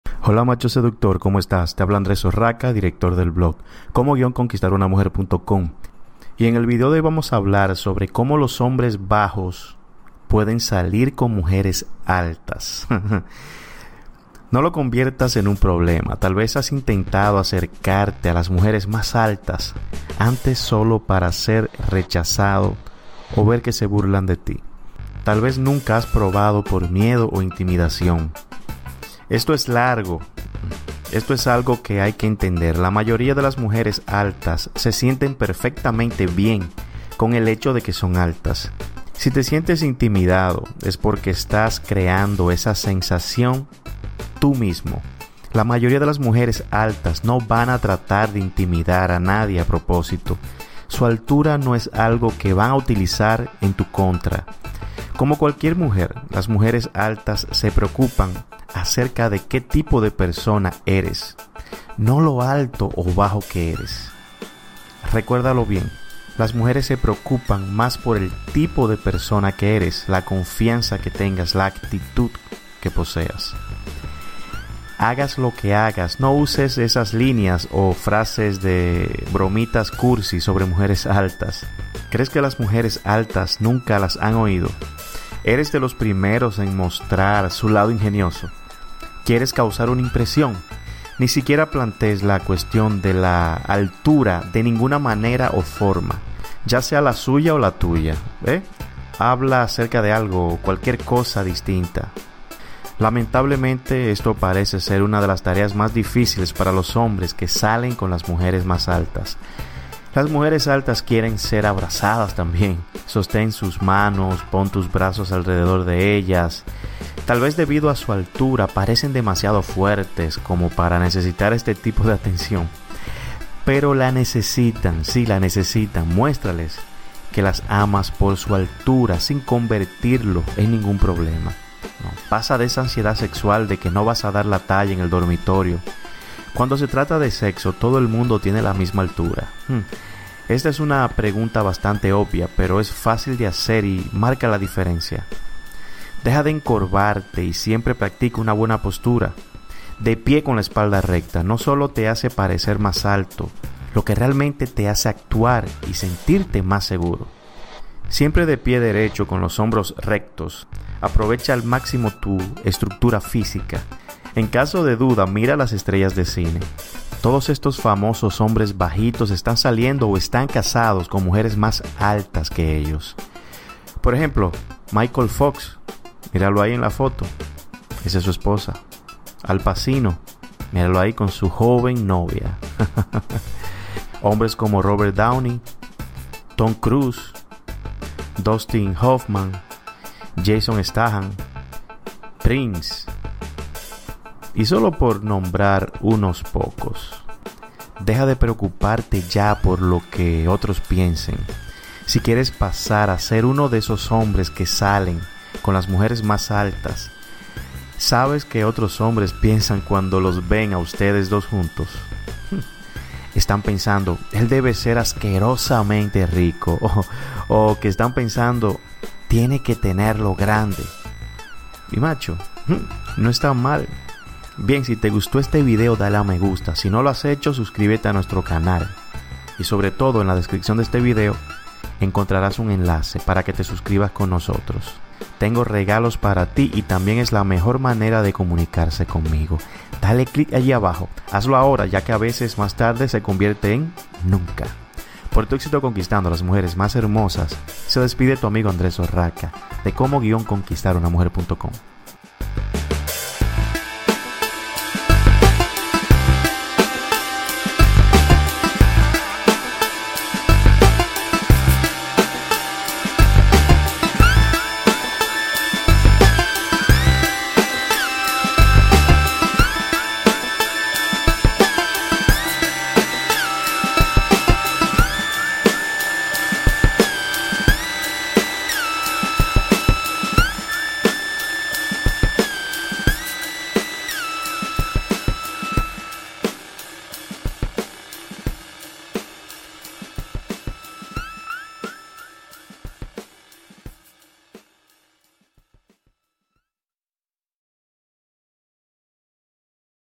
Cómo Los Hombres Bajos Pueden Salir Con Mujeres Altas- Mujer Mas Alta Que El Hombre En Una Relación
SI Una Mujer Es Mas Alta Que El Hombre En Una Relación, Que Pasa?

Pues todo depende de ti, las mujeres no prestan GRAN atención a tu aspecto, sino mas bien a como eres, como la haces sentir!

Una mujer alta con un hombre bajo es algo normal, lo ves en las parejas de actores de cine!

En este video te enseñare algunos, como por ejemplo Tom Cruise!

Apropósito, cuanto mide Tom Cruise?

1.7 metros o en pies serian 5.6

Si, el lleva una relación de pareja con una mujer alta siendo el un hombre bajo, y son casi todas sus mujeres.

Es por eso que no puedes tener dudas de que una mujer alta con hombre pequeño puede ser felices!

Eso lo podras comprobar en la calle, o acaso no has visto a mujeres altas con novios bajitos? Claro que si!

Quítate de la cabeza eso de que las chicas altas con chicos bajos jamas podrán ser!